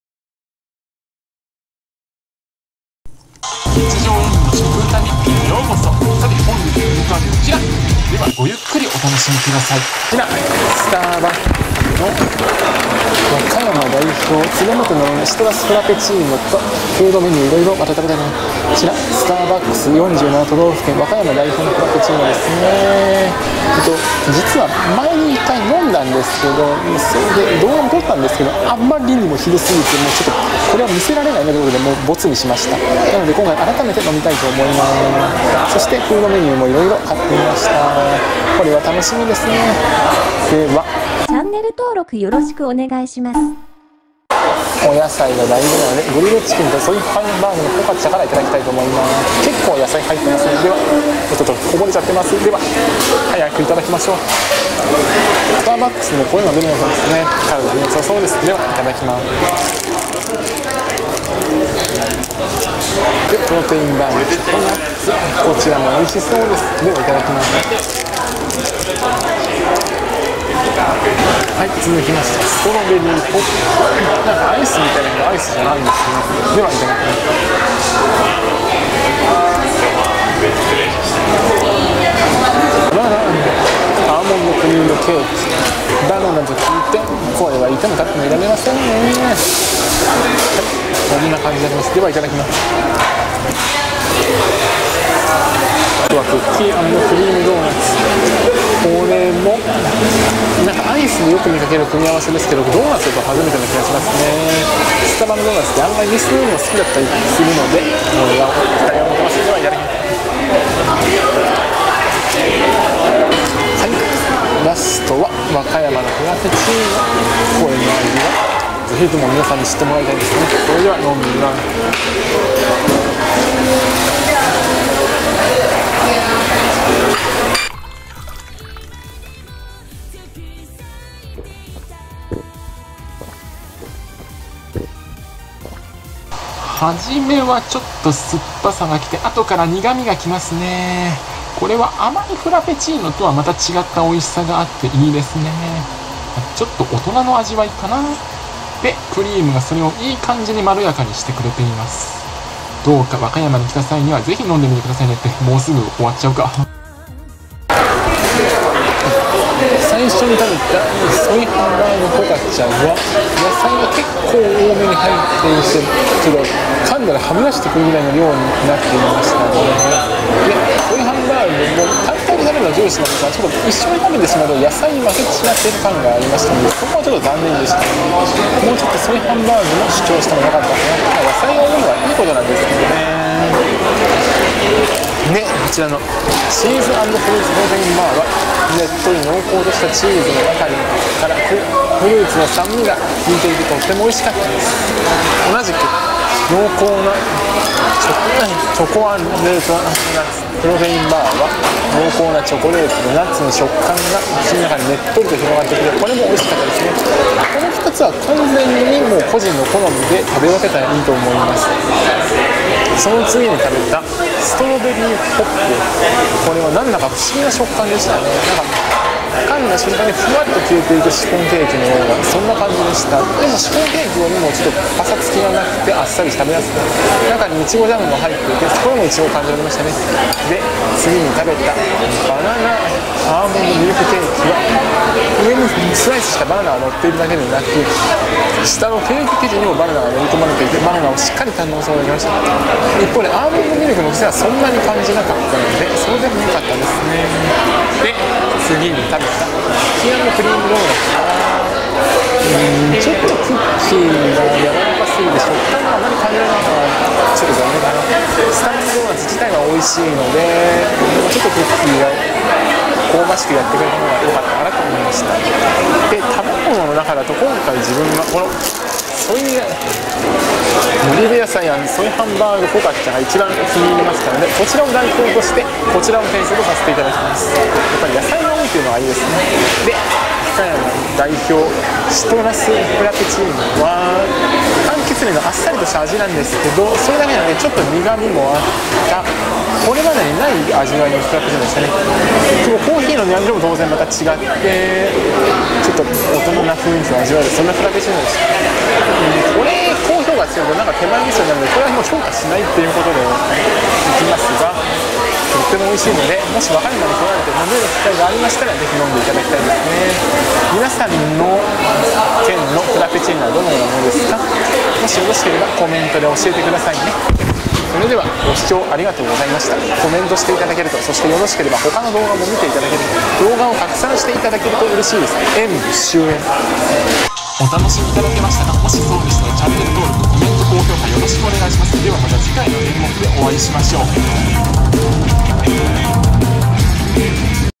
スタバの47都道府県限定フラペチーノ和歌山県代表とフード色々食べてみた。スタバ
スタバの都道府県限定フラペチーノが気になる方、自分は和歌山県代表フラペチーノ「和歌山つれもてのもらみかんシトラスフラペチーノ」とフードメニュー色々を食べて見ました。その感想は動画内で！！